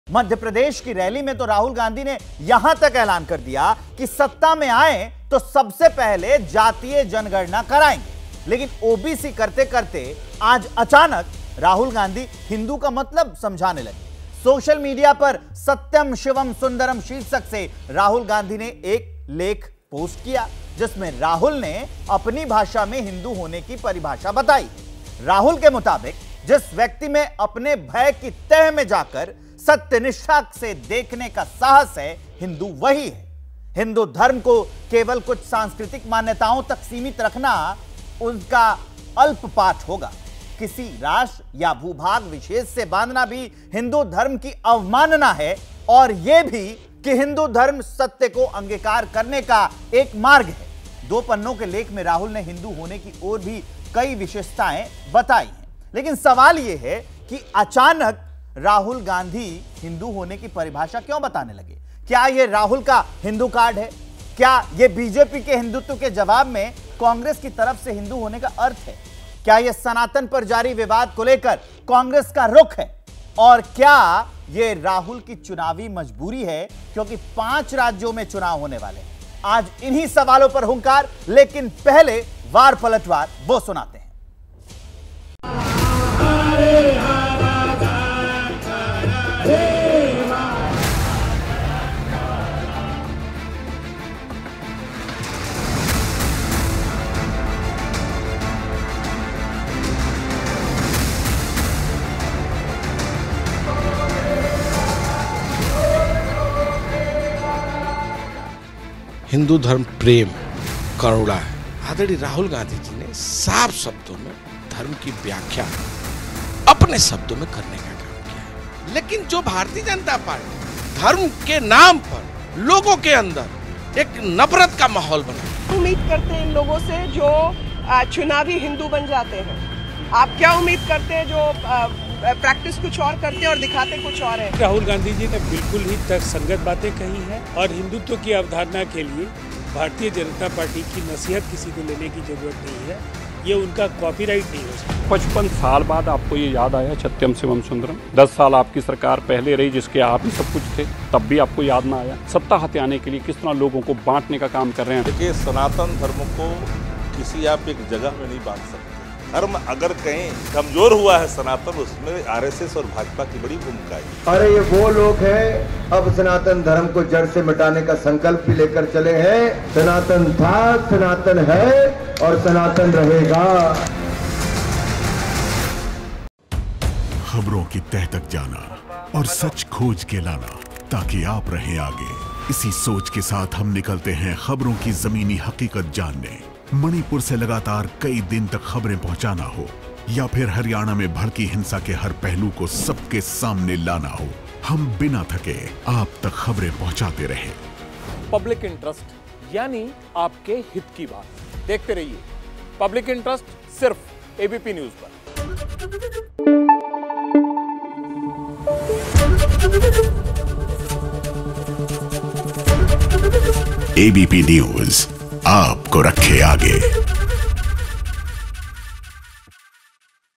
Rahul Gandhi: अब कांग्रेस नेता राहुल गांधी ने हिंदुत्व को लेकर क्या कह दिया ! | Hoonkar
Rahul Gandhi Satyam Shivam Sundaram: कांग्रेस नेता राहुल गांधी ने सत्यम शिवम सुंदरम हेडिंग के साथ एक लेख की तस्वीर सोशल मीडिया पर डाली है, जिस पर यूजर्स खूब प्रतिक्रिया दे रहे हैं. हिंदुत्व की विचारधारा और इसमें निहित करुणा, प्रेम, त्याग, दया को रेखांकित करते लेख के एक हिस्से को भी उन्होंने माइक्रो ब्लॉगिंग साइट एक्स (पूर्व में ट्विटर) पर डाला है. बाकी हिंदी और अंग्रेजी में दो-दो पन्ने का दस्तावेज उन्होंने एक्स पर शेयर किया है.